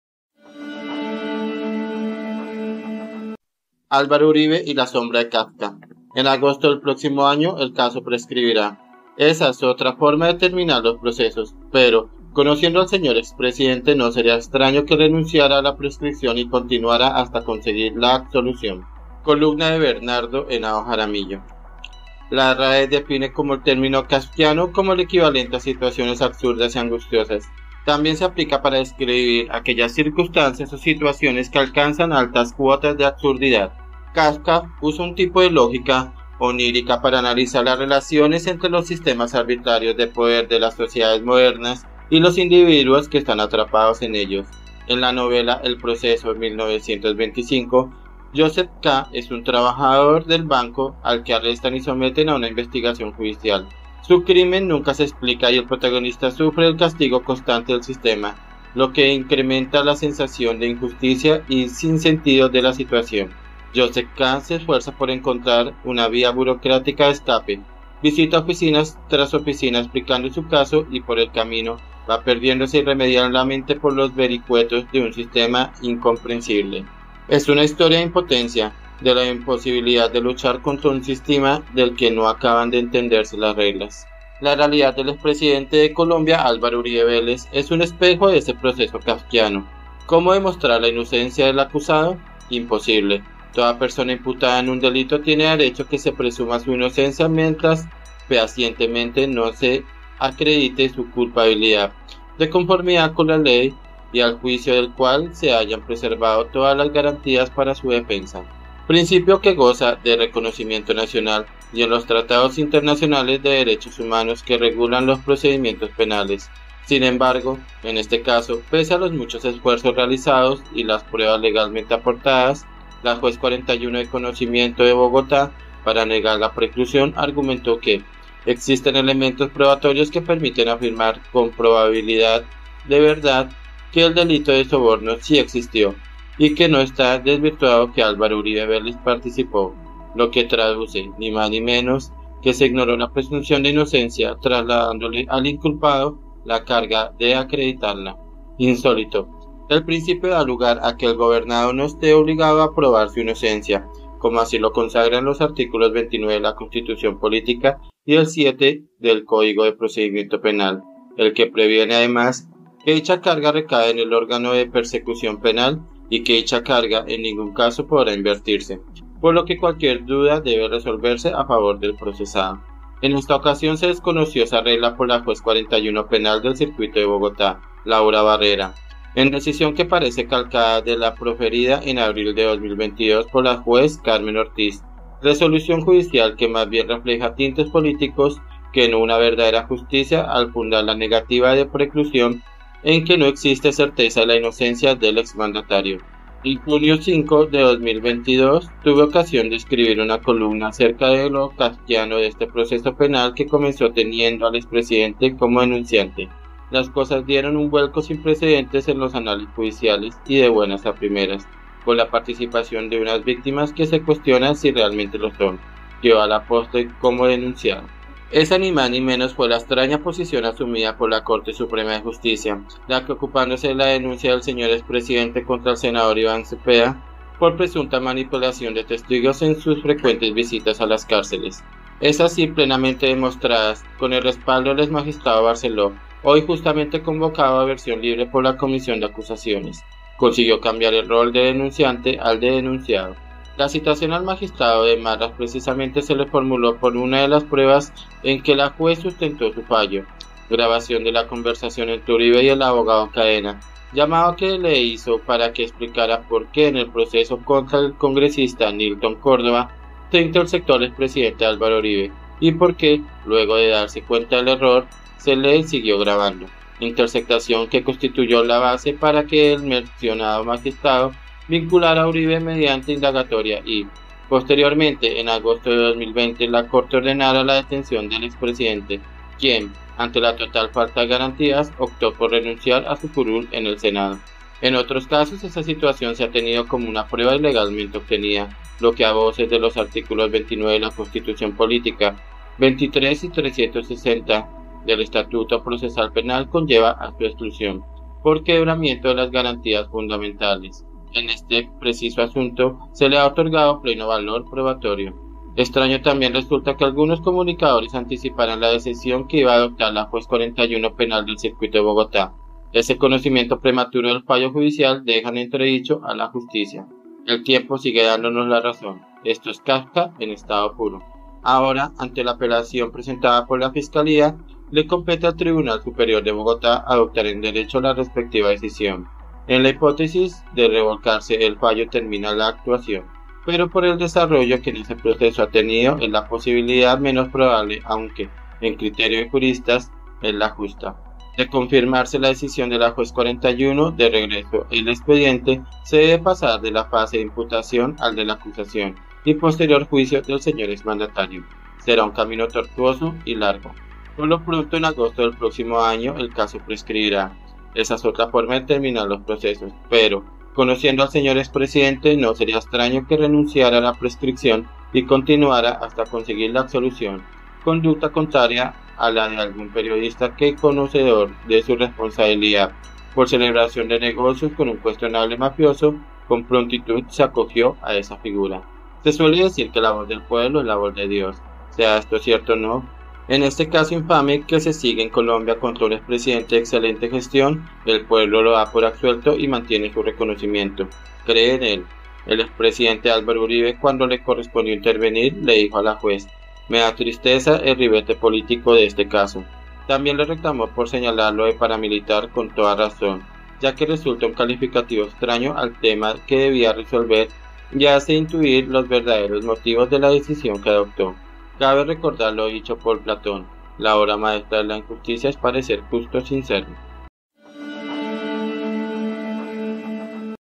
ÁLVARO URIBE Y LA SOMBRE DE KAFKA 🚨 Por Bernardo Henao Jaramillo
En agosto del próximo año el caso prescribirá. Esa es otra forma de terminar los procesos. Pero, conociendo al señor expresidente, no sería extraño que renunciara a la prescripción y continuara hasta conseguir su absolución
columna Bernardo Henao Jaramillo